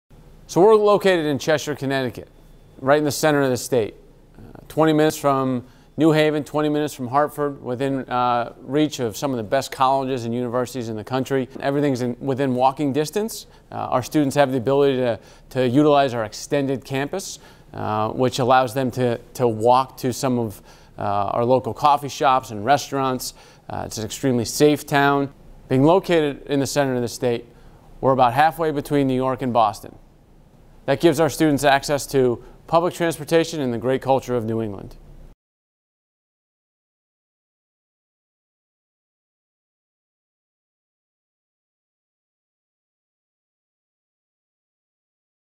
FAQ: Where is Cheshire, Connecticut and what's it like?
Admission Associate Dan Mehleisen discusses Cheshire Academy's great location and all its benefits in our Frequently Asked Questions series.